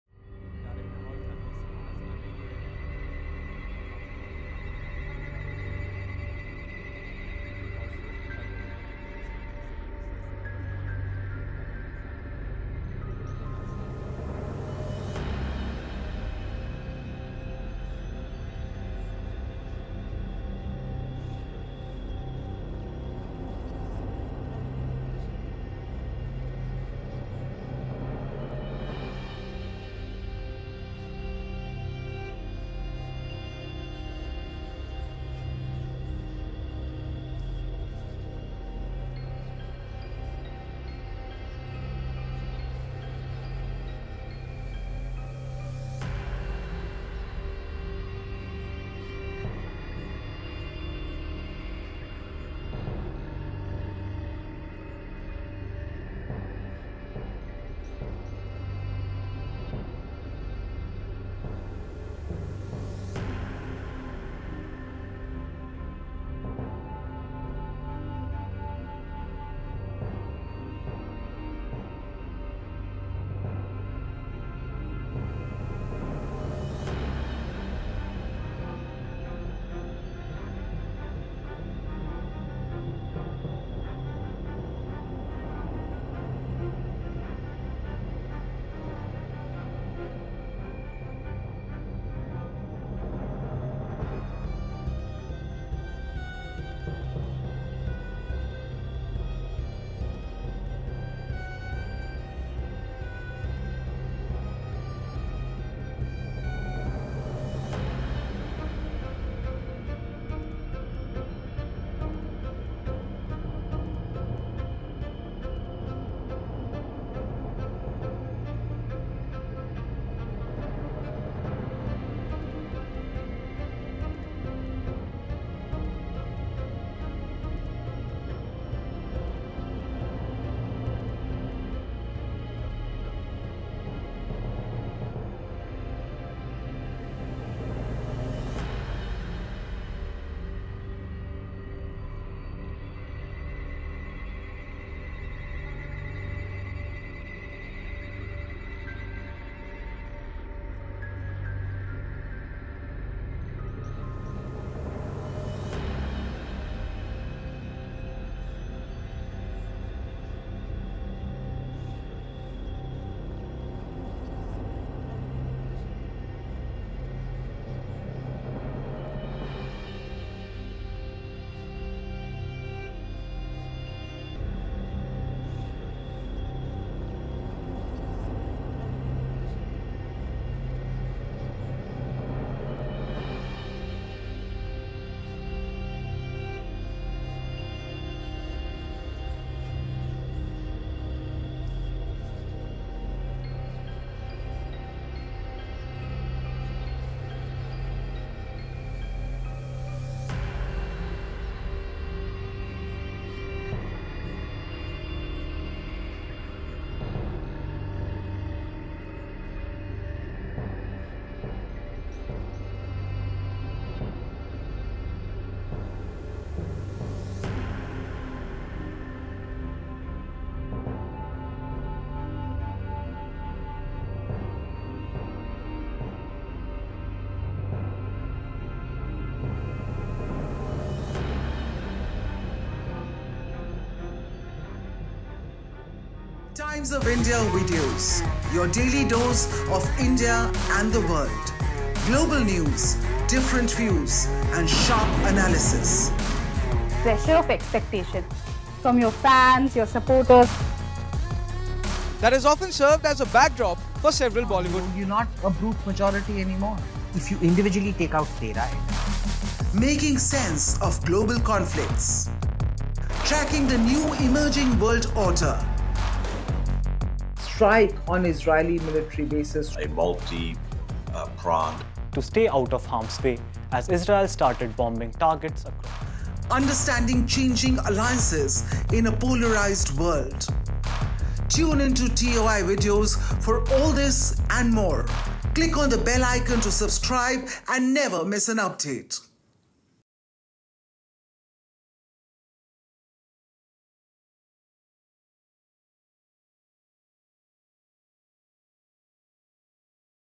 Iran Leader Khamenei's Stunning Offer To Trump After U.S. Victory; 'Bitter Policies...' | Watch
Iran has called Donald Trump's victory in the 2024 U.S. elections an opportunity to reassess past U.S. policies. Iranian Foreign Ministry spokesperson Esmail Baghaei stated that Trump's win offers a chance to review the "wrong policies" that have shaped relations between the two nations. Watch for more details.